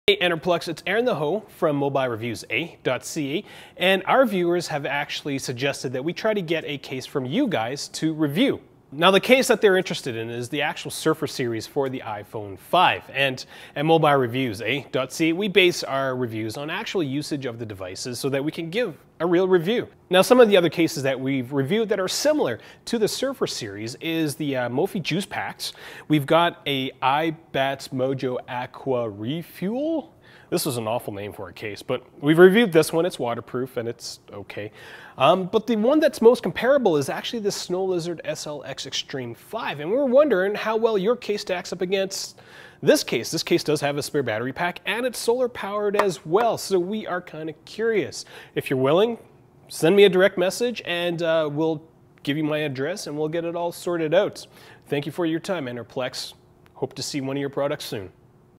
Enerplex, will you give us a Surfr Series Case for the iPhone 5S to review?
One of our viewers suggested that we review the Surfr Series of battery/solar powered cases by Enerplex. It looks like an awesome case.

We're a growing review channel and we're curious how the Surfr Series holds up against other cases that we've reviewed such as the Mophie Juice Pack, iBattz Mojo Aqua Refuel and Snow Lizards SLXtreme 5.